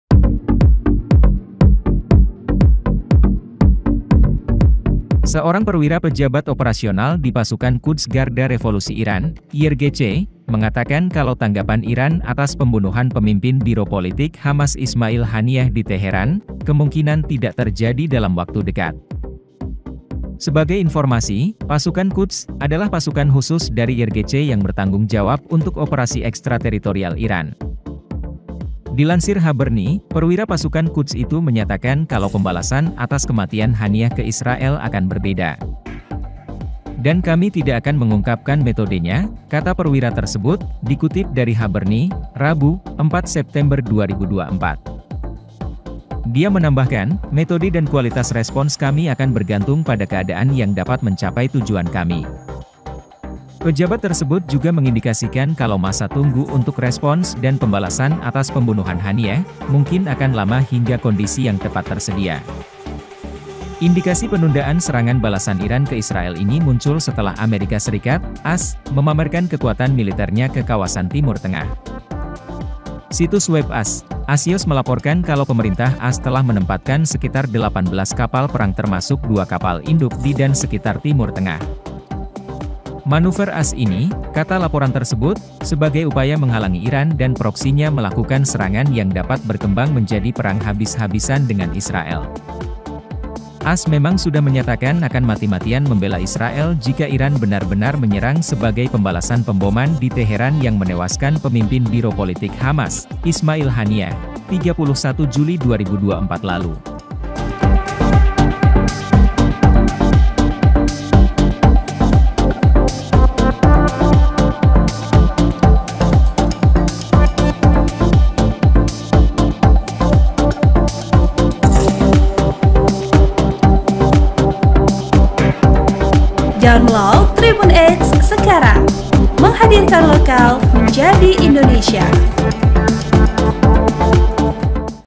AS Pamer Kapal Perang, Iran Kembali Mundur Waktu Serangan ke Israel
POS-KUPANG.COM - Seorang perwira pejabat operasional di Pasukan Quds Garda Revolusi Iran (IRGC) mengatakan kalau tanggapan Iran atas pembunuhan pemimpin biro politik Hamas Ismail Haniyeh di Teheran, kemungkinan tidak terjadi dalam waktu dekat.

Sebagai informasi, Pasukan Quds,adalah pasukan khusus dari IRGC yang bertanggung jawab untuk operasi ekstrateritorial Iran.

Dilansir Khaberni, Perwira Pasukan Quds itu menyatakan kalau pembalasan atas kematian Haniyeh ke Israel akan berbeda.(POS-KUPANG.COM, Tribunnews.com)

Artikel ini telah tayang di

Source   : Tribunnews.com

Penulis  : Hasiolan Eko P Gultom/Tribunnews.com